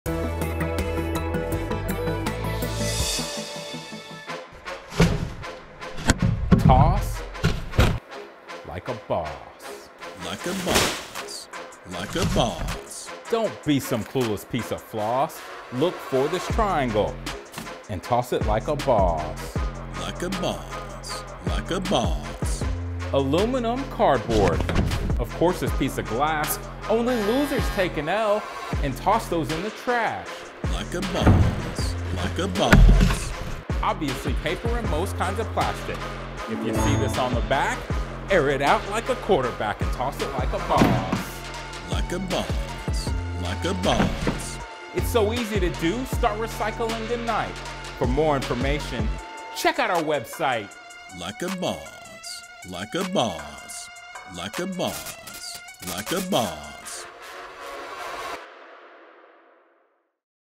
🏆 Toss Like a Boss
3CMA Award of Excellence & TATOA PSA with Best Use of Humor
Recycling is easier than you think. and learn how to "Toss Like a Boss!"